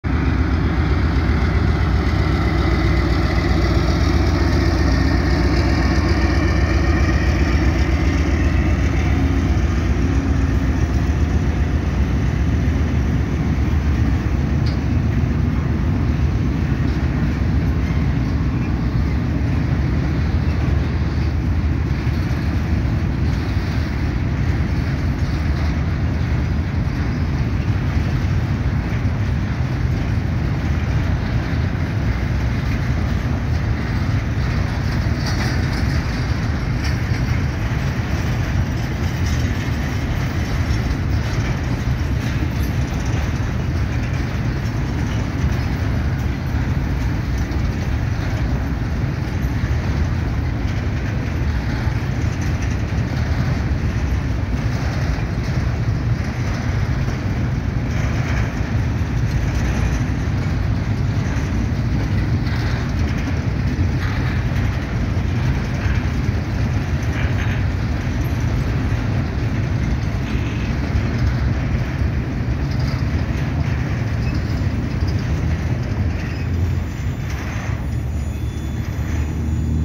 LINDO TREN DEL(TAC) PASANDO POR COLONIA CAROYA cba... RUMBO AL NORTE
locomotoras : CRRC Nº9766 mas 27 tolvas graneras
fecha de filmacion : 07/05/2024
hora : 13:20
autor material : MARIANO TRENES